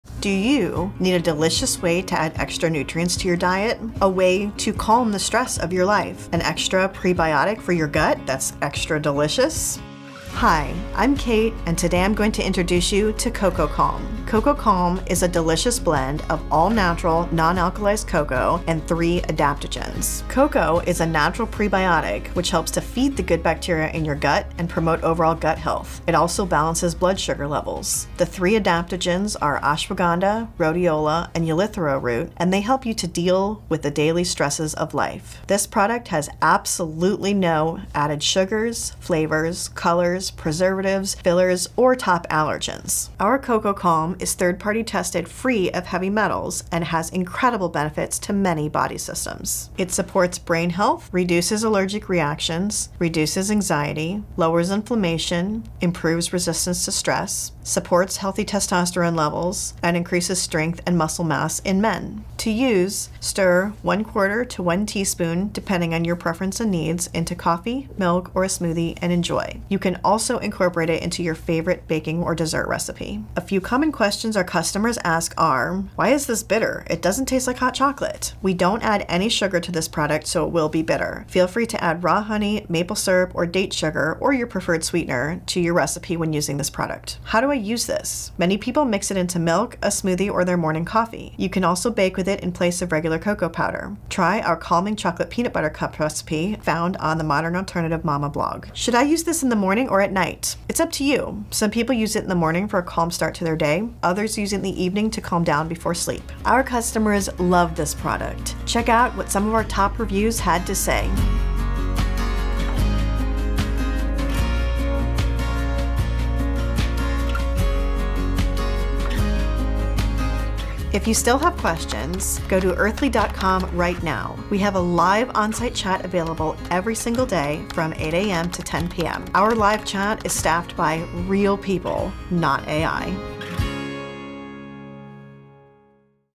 Product Overview: Cocoa Calm
In todays product overview we will be talking about Cocoa Calm.  This amazing product is a healthy, calming way to satisfy your sweet tooth.  It is perfect for hot cocoa, baking, and more!  Follow along in this video to learn more.